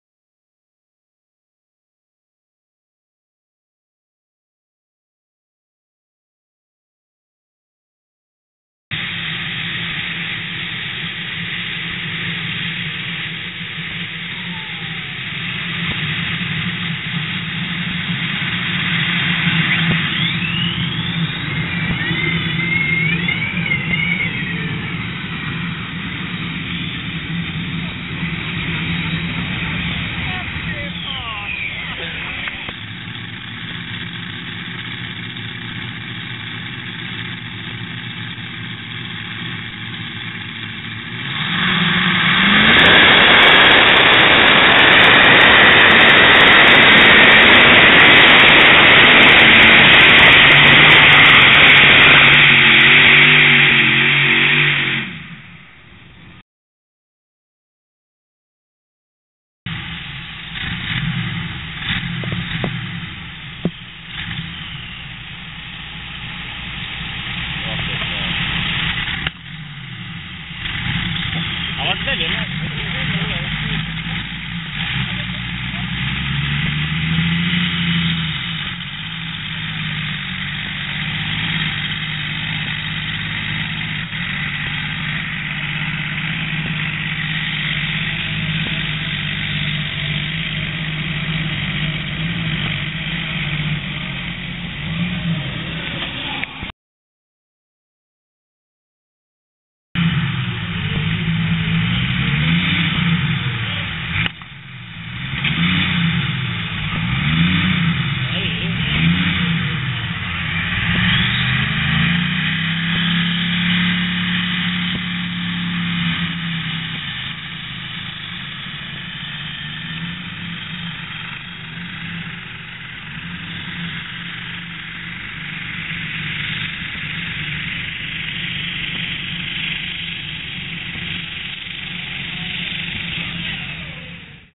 Tractor Pulling Trøgstad, Norway 2005
This is a compilation of old video and picture files from a pocket camera from that time, but it is still some memory from the past with insane machines.